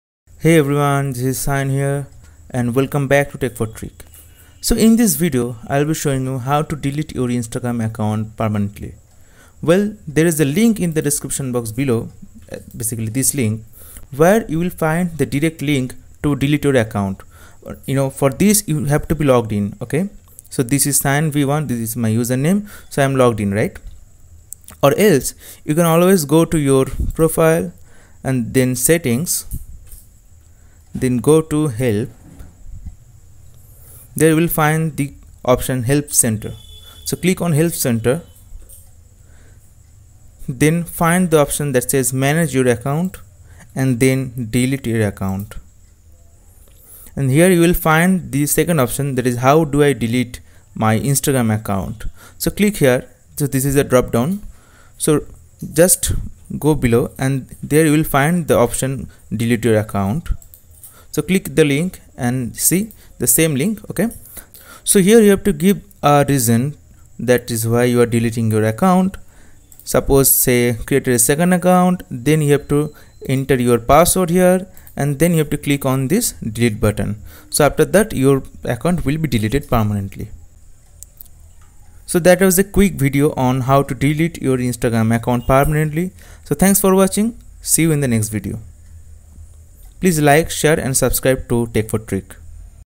How to Delete Instagram Account Permanently Easily | New Trick | Tec4Tric
* You need to be logged in or log in with your account and copy and paste the link to delete your Instagram account in just one click.